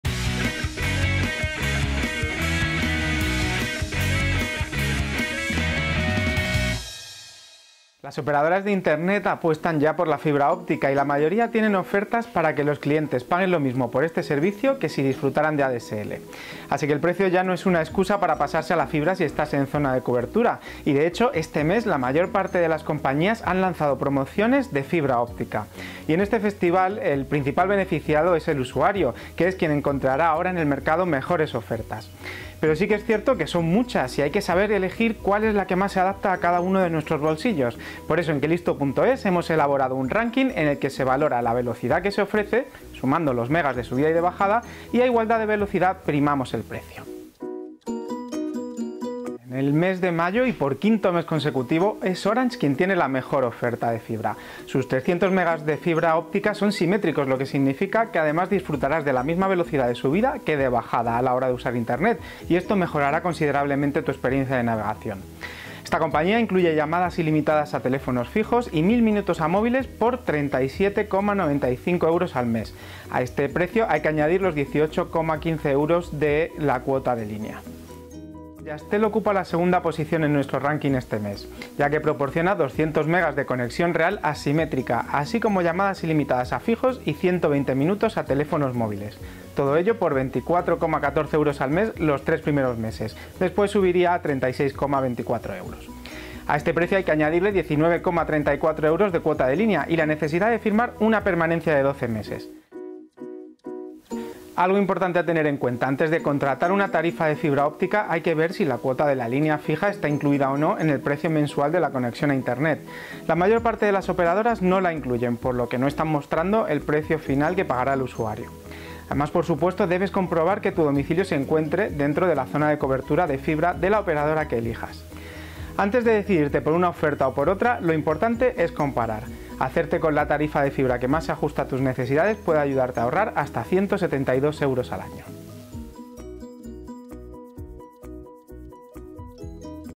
Tarifas de Fibra Óptica más baratas 2017 | COMPARATIVA
🔎🔎 ¿Quieres saber cual es la tarifa de fibra óptica que mejor se adapta a ti? Descubrela ¡AQUÍ!

▽ Transcripción del vídeo ▽

Las operadoras de telecomunicaciones apuestan por la fibra óptica y la mayoría tienen ofertas para que los clientes paguen lo mismo por este servicio que si disfrutaran de ADSL. El precio ya no es excusa para pasarse a la fibra si estás en zona de cobertura.

Este mes la mayor parte de las compañías han lanzado promociones de fibra óptica. En este festival, el principal beneficiado es el usuario, que encontrará ahora en el mercado mejores ofertas. Como son muchas y hay que saber elegir cuál es la que más se adapta a cada bolsillo, en Kelisto.es elaborado un ranking en el que se valora la velocidad que se ofrece (sumando los Mb de subida y de bajada) y, a igualdad de velocidad, el precio.

La mejor oferta: Orange
Orange, por quinto mes consecutivo, tiene la mejor oferta. Sus 300 Mb de fibra óptica son simétricos, lo que significa que disfrutarás de la misma velocidad de bajada que de subida a la hora de usar Internet, por lo que tu experiencia de navegación mejorará considerablemente.

Además, la compañía incluye llamadas ilimitadas a fijos y 1.000 minutos a móviles por 37,95 euros al mes, a los que hay que añadir los 18,15 euros de cuota de línea. Por si fuera poco, los tres primeros meses rebaja su precio a 28,95 euros. Eso sí, requiere un año de permanencia.

Como primamos tanto la velocidad de subida como la de bajada, Jazztel ocupa la segunda posición en nuestro ranking, ya que proporciona 200 Mb de conexión real asimétrica, así como llamadas ilimitadas a fijos y 120 minutos a teléfonos móviles por 24,14 euros al mes los tres primeros meses, después subiría a 36,24 €. Hay que añadirle 19,34 euros de cuota de línea y la necesidad de firmar una permanencia de 12 meses.

Además, ahora mismo cuenta con una promoción que permite disfrutar de un smartphone nuevo por 0 euros y un mes de conexión gratis.

A tener en cuenta

Antes de contratar una tarifa de fibra óptica hay que ver si la cuota de la línea fija está incluida o no en el precio mensual de la conexión a Internet. La mayor parte de las operadoras no la incluyen, por lo que no están mostrando el precio final que pagará el usuario. Y por supuesto, debes comprobar que tu domicilio se encuentre dentro de la zona de cobertura de fibra de la operadora que elijas.

Antes de decidirte por una oferta u otra lo importante es comparar, hacerte con la tarifa de fibra que más se ajusta a tus necesidades puede ayudarte a ahorrar hasta 172 euros al año.